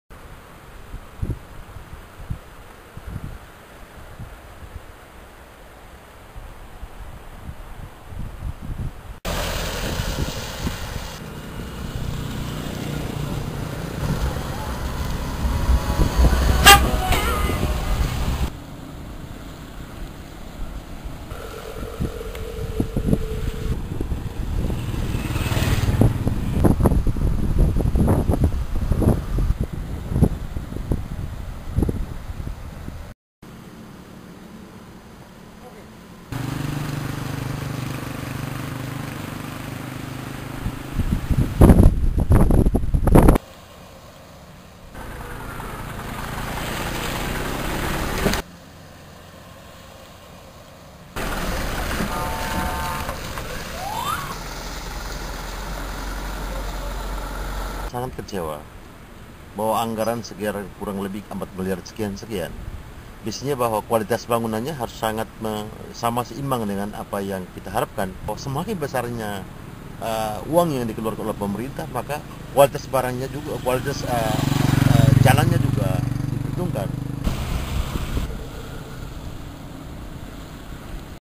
Jalan Lintas Indonesia Timor Leste Terancam Putus
Metrotvnews.com, Belu: Hujan deras yang mengguyur daratan Pulau Timor sejak beberapa pekan terakhir mengakibatkan jalur Trans Nusa di Desa Naekasa Kabupaten Belu, Nusa Tenggara Timur menuju negara Timor Leste terancam putus padahal jalur ini baru dikerjakan dan masih dalam masa pemeliharaan, Selasa (13/1/2015).